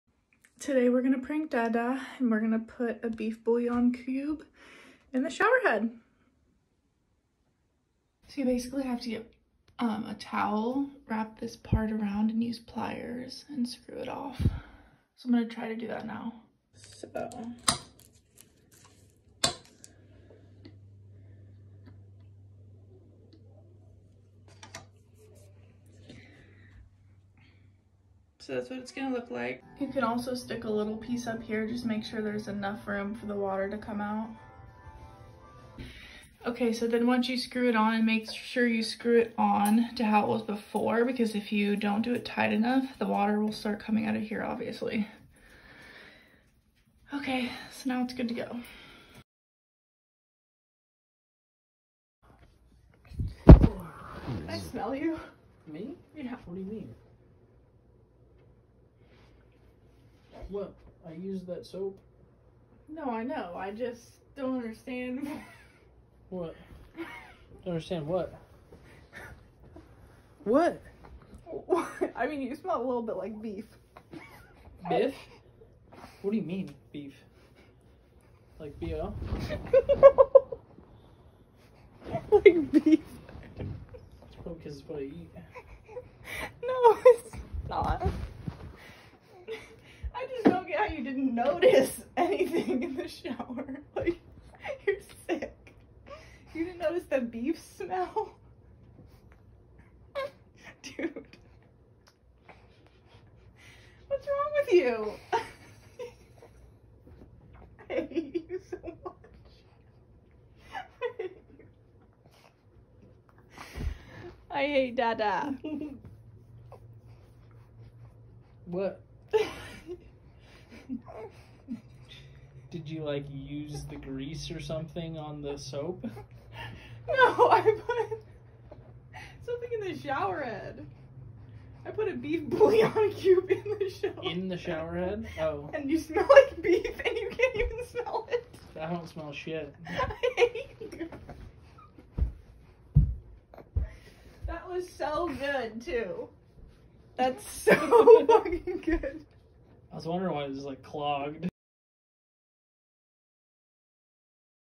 Beef bouillon cube shower prank
Shower prank with beef bouillon cube. Funny fail. It is hard to get him!
Update: It definitely smells like beef when you turn the shower on🤢